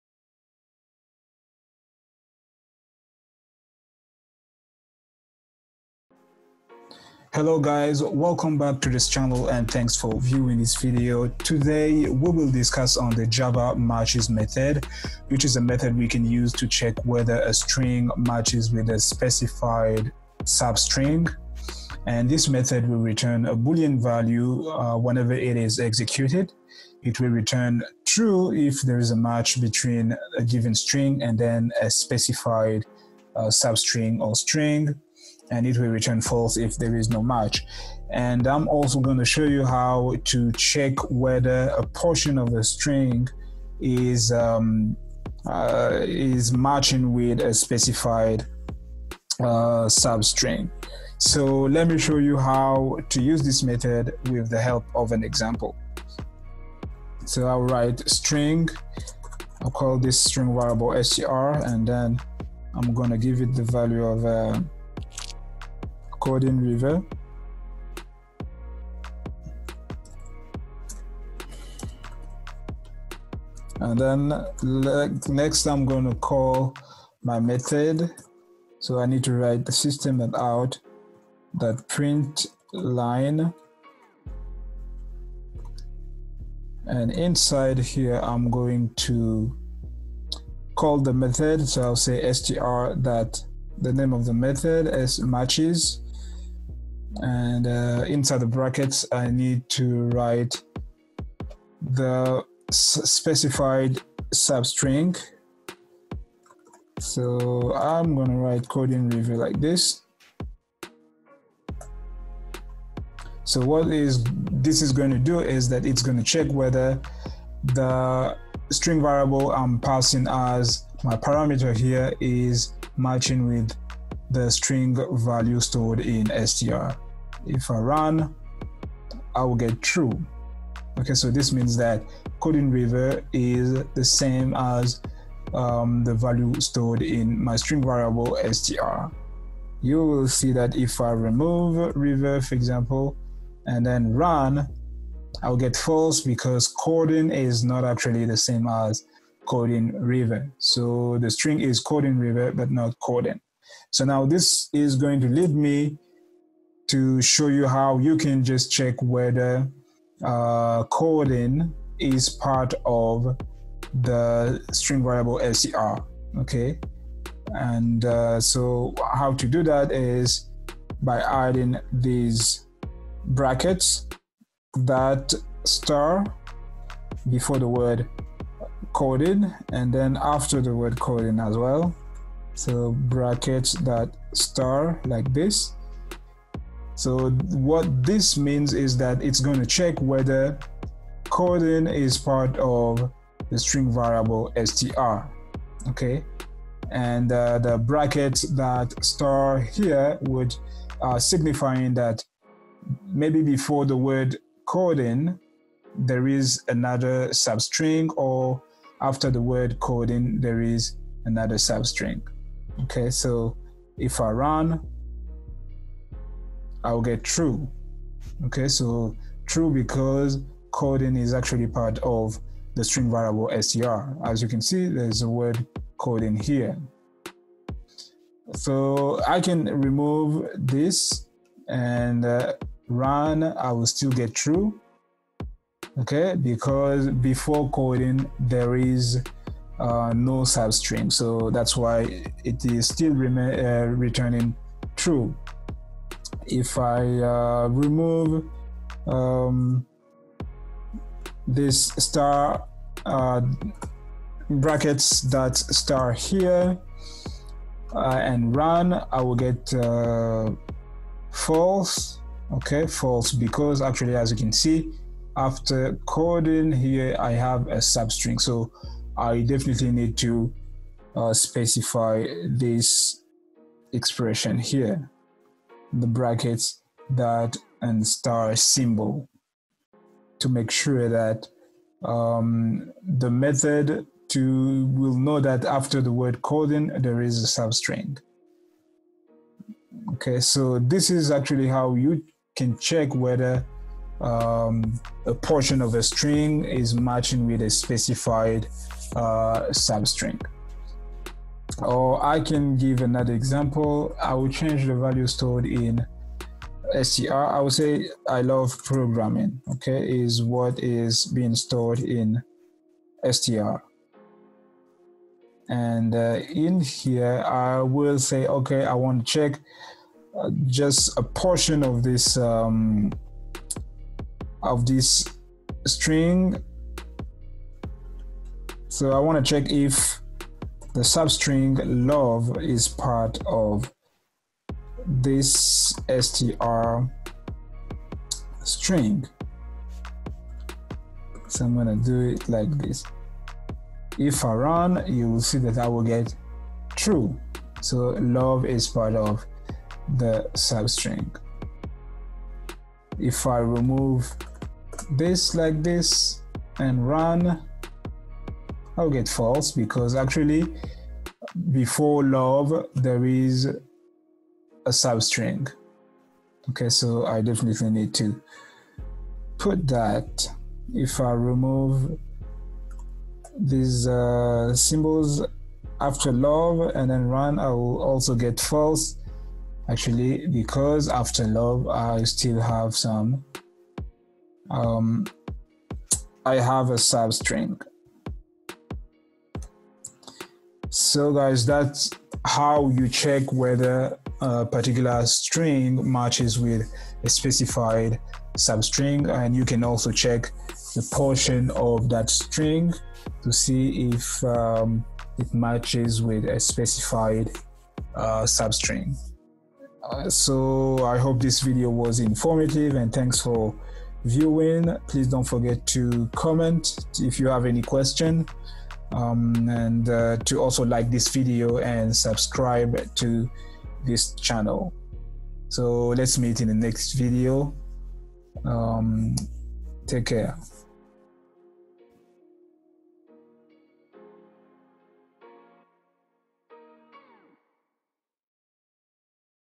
How to check whether a String matches with a specified substring using the Java matches() method
In this video, we will discuss on the Java matches() method, which can be used to check whether a String matches with a specified regular expression. This method returns a Boolean value whenever it is executed. It returns true if there is a match and false if there is no match.
Let me show you how to use this method with the help of an example

String str = "Coding River";
System.out.println(str.matches("Coding River"));
This statement will return "true"

System.out.println(str.matches("Coding"));
This statement will return false

System.out.println(str.matches("(.*)Coder(.*)"));
This statement will return false, because we have changed the characters, so they are not matching a String any more